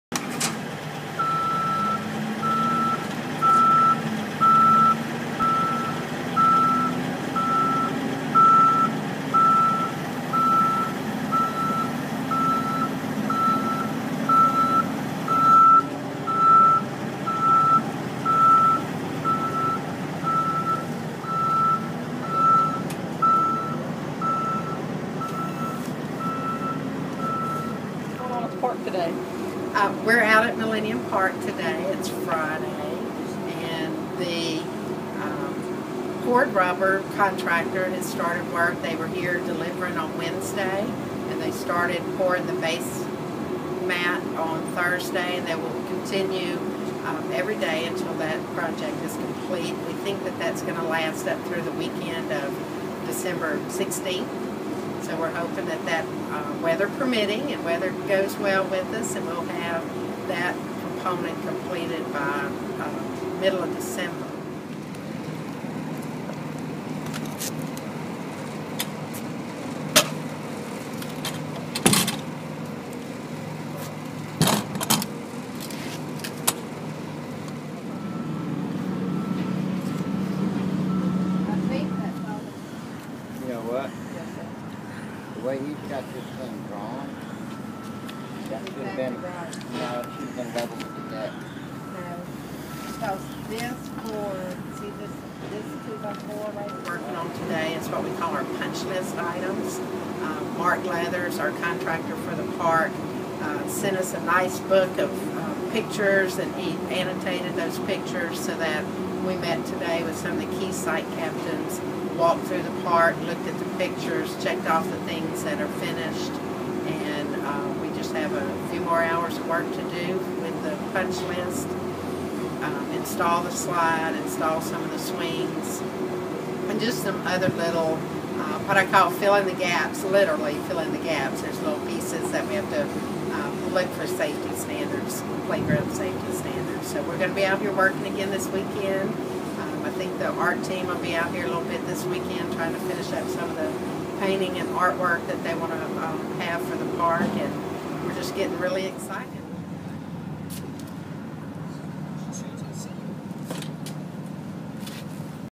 Millennium Park Rebuild
Lori Marinovich with Lakefront Development talks with the American Press about the progress of the Millennium Park rebuild on Friday, Dec. 2. Marinovich said workers are now pouring the rubber flooring. The flooring could be done by the middle of the month.